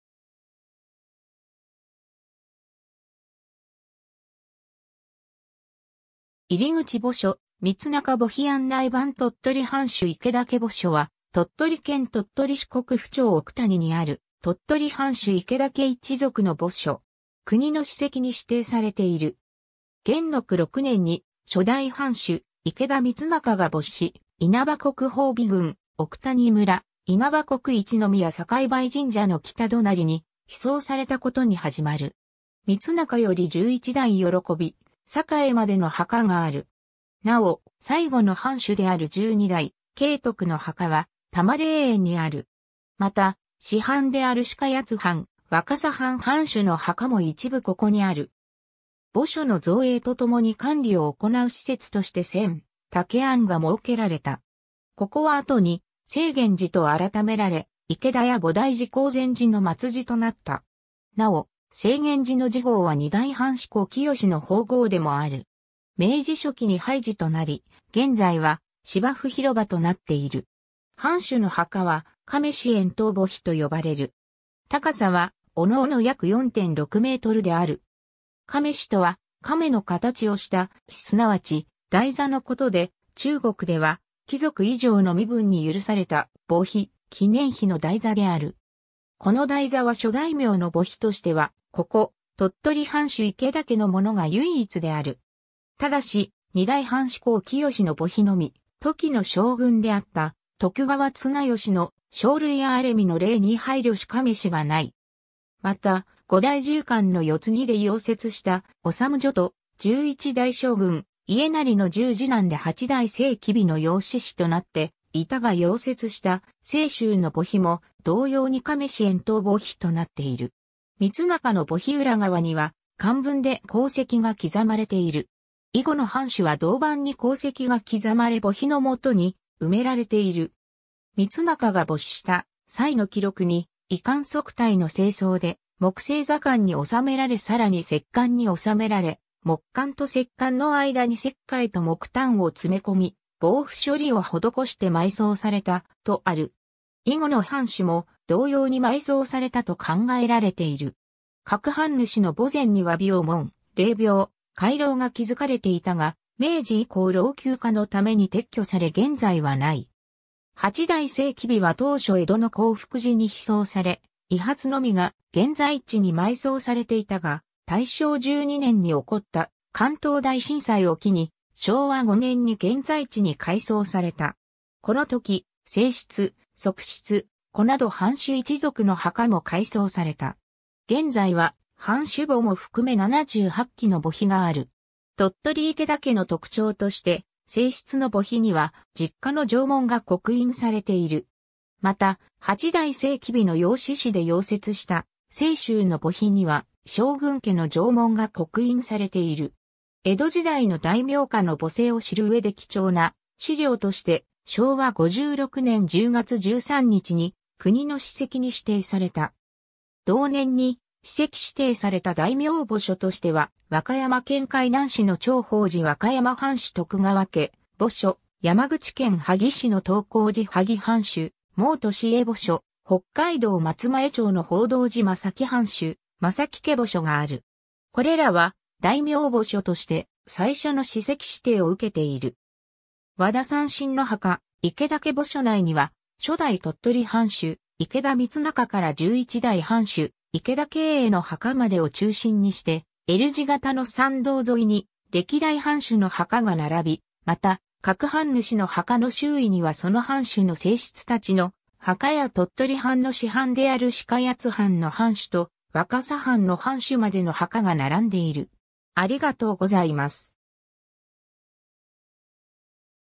鳥取藩主池田家墓所
入口 墓所（左奥が光仲墓） 光仲墓碑 案内板 鳥取藩主池田家墓所（とっとりはんしゅいけだけぼしょ）は、鳥取県鳥取市国府町奥谷にある鳥取藩主池田家一族の墓所。
 元禄6年（1693年）に初代藩主池田光仲が没し、因幡国法美郡奥谷村、因幡国一宮宇倍神社の北隣に被葬されたことに始まる。
光仲より11代慶栄までの墓がある。
なお、最後の藩主である12代慶徳の墓は多磨霊園にある。
また、支藩である鹿奴藩（東館）、若桜藩（西館）藩主の墓も一部ここにある。
 墓所の造営と共に管理を行う施設として千岳庵が設けられた。
ここは後に清源寺と改められ池田家菩提寺興禅寺の末寺となった。
なお、清源寺の寺号は2代藩主綱清の法号でもある。
明治初期に廃寺となり、現在は芝生広場となっている。
 藩主の墓は亀趺円頭墓碑と呼ばれる。
高さは各々約4.6メートルである。
亀趺（きふ）とは亀の形をした「趺」すなわち台座のことで、中国では貴族以上の身分に許された墓碑・記念碑の台座である。
この台座は諸大名の墓碑としては、ここ鳥取藩主池田家のものが唯一である。
ただし、2代藩主綱清の墓碑のみ、時の将軍であった徳川綱吉の「生類憐れみの令」に配慮し亀趺が無い。
また、5代重寛の世嗣で夭折した治恕と、11代将軍家斉の十二男で8代斉稷の養嗣子となっていたが夭折した斉衆の墓碑も同様に亀趺円頭墓碑となっている。
 光仲の墓碑裏側には漢文で功績が刻まれている。
以後の藩主は銅板に功績が刻まれ墓碑の下に埋められている。
 光仲が没した際の記録に、衣冠束帯の正装で木製座棺に収められ更に石棺に収められ、木棺と石棺の間に石灰と木炭を詰め込み防腐処理を施して埋葬された、とある。
以後の藩主も同様に埋葬されたと考えられている。
 各藩主の墓前には廟門・霊廟・回廊が築かれていたが、明治以降老朽化のために撤去され現在はない。
 8代斉稷は当初江戸の弘福寺に被葬され遺髪のみが現在地に埋葬されていたが、大正12年（1923年）に起こった関東大震災を機に、昭和5年（1930年）に現在地に改葬された。
この時、正室・側室・子など藩主一族の墓も改葬された。
現在は藩主墓も含め78基の墓碑がある。
鳥取池田家の特徴として正室の墓碑には実家の定紋が刻印されている。
また、8代斉稷の養嗣子で夭折した斉衆の墓碑には将軍家の定紋（三葉葵）が刻印されている。
 江戸時代の大名家の墓制を知る上で貴重な資料として、昭和56年（1981年）10月13日に、国の史跡に指定された。
同年に史跡指定された大名墓所としては和歌山県海南市の長保寺和歌山藩主徳川家墓所・山口県萩市の東光寺萩藩主毛利家墓所・北海道松前町の法幢寺松前藩主松前家墓所がある。
これらは大名墓所として最初の史跡指定を受けている。
 和田三信の墓 池田家墓所内には初代鳥取藩主池田光仲から11代藩主池田慶栄の墓までを中心にしてL字型の参道沿いに歴代藩主の墓が並び、また、各藩主の墓の周囲にはその藩主の正室達の墓や鳥取藩の支藩である鹿奴藩の藩主(初代から8代目まで)と若桜藩の藩主(初代から7代目)までの墓が並んでいる。